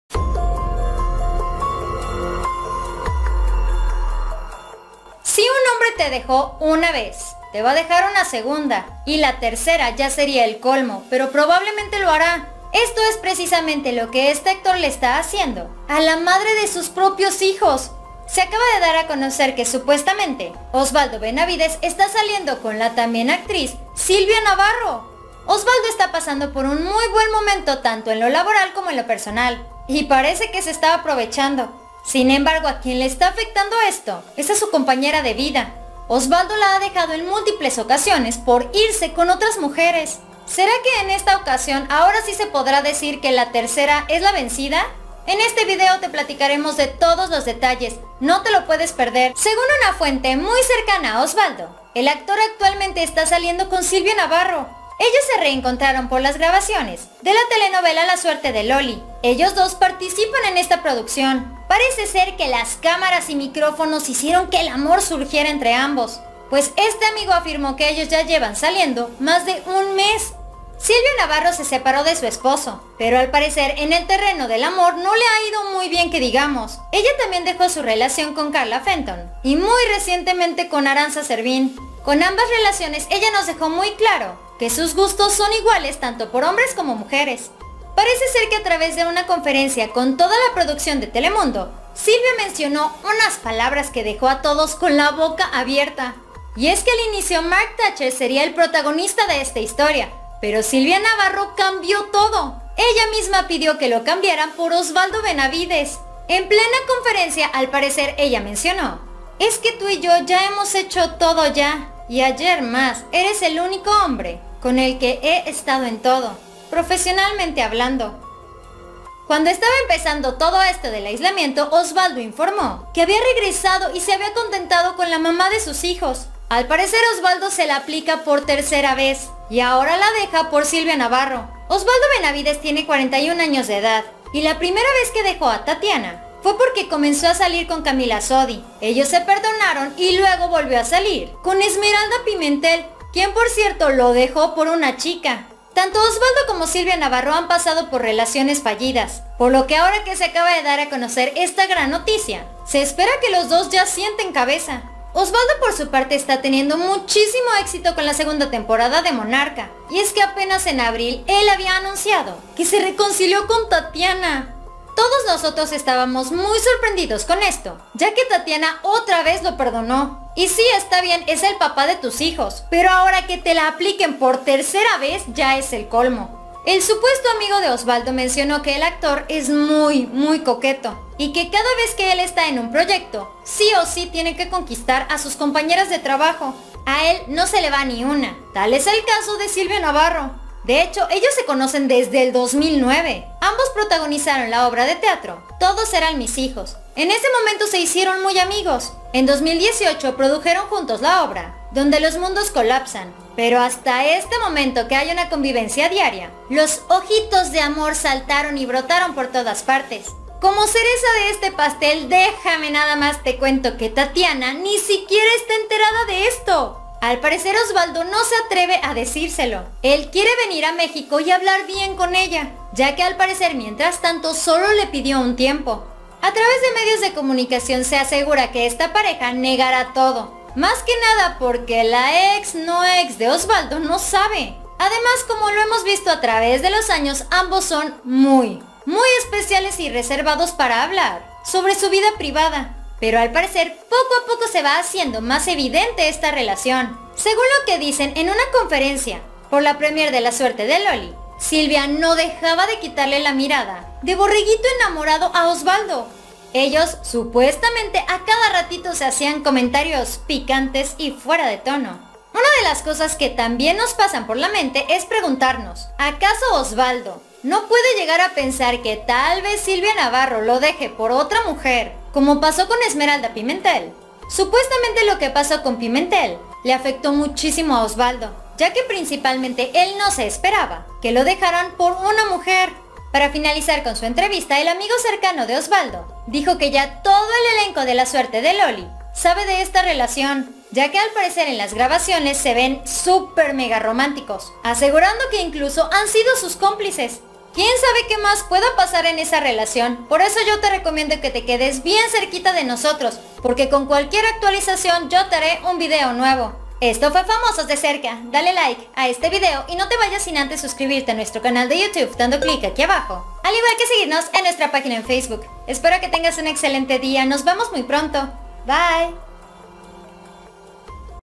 🚨😱 ¡ Osvaldo Benavides 𝗔𝗯𝗮𝗻𝗱𝗼𝗻𝗮 𝗛𝗶𝗷𝗼𝘀 𝘆 𝗘𝘀𝗽𝗼𝘀𝗮 por Silvia Navarro ! ⛔💥
Poco le importo Dejar a su familia por tercera vez a Osvaldo Benavides

 BIENVENIDOS!!!